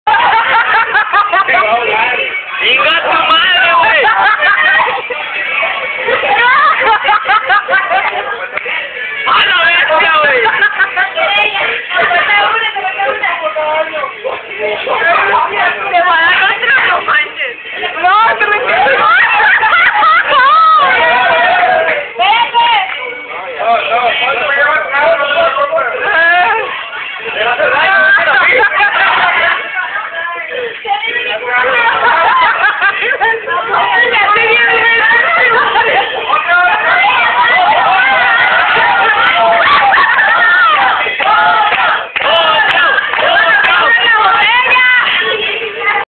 Feliz cumple mirelle 2
segundo año de tu cumpleaños amiga, que buena comida verdad o bueno la mareada que te puso el mesero de los brasotes